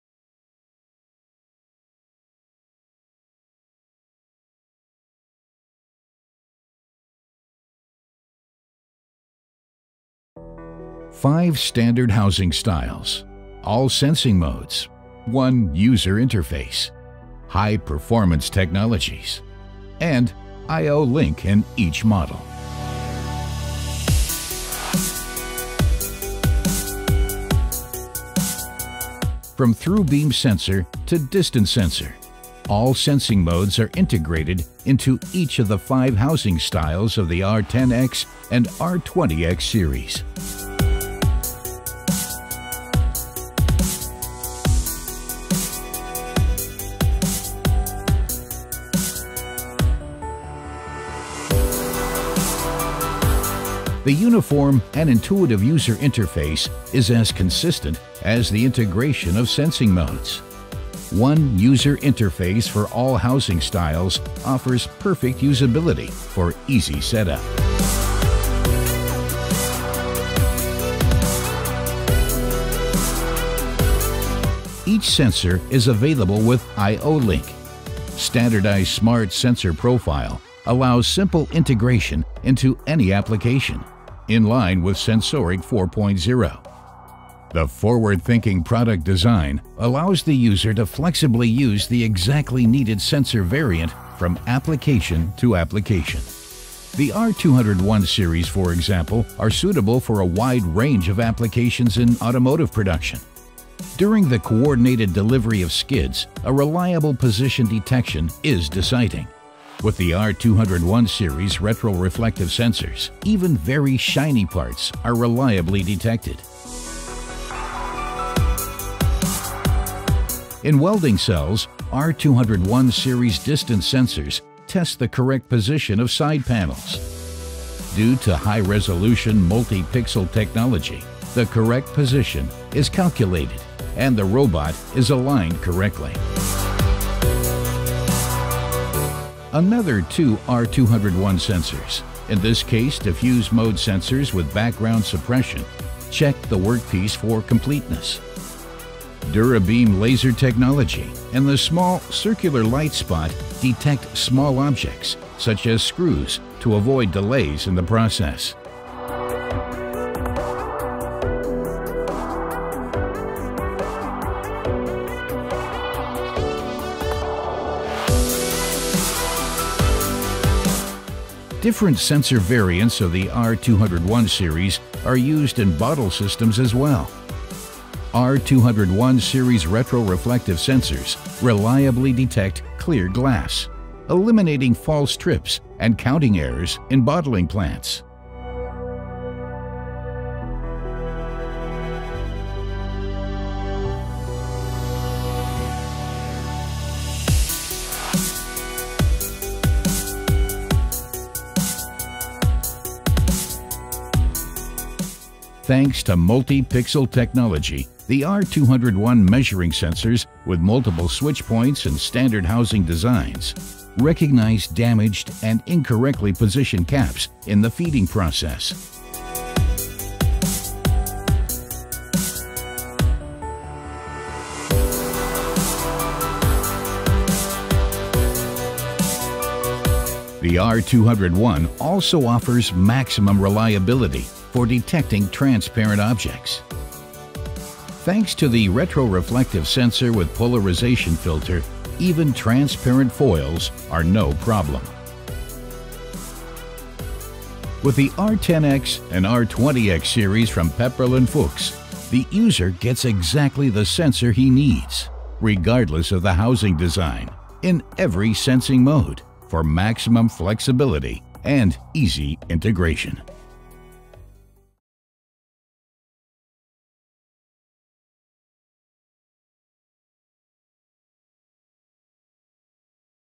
R10x and R20x Series | Photoelectric sensors with forward-thinking product design
– From thru-beam sensor to distance sensor, all sensing modes are integrated into each of the five housing styles of the R10x and the R20x series. The uniform and intuitive user interface is as consistent as the integration of sensing modes: One user interface for all housing styles and sensing modes offers perfect usability for easy setup.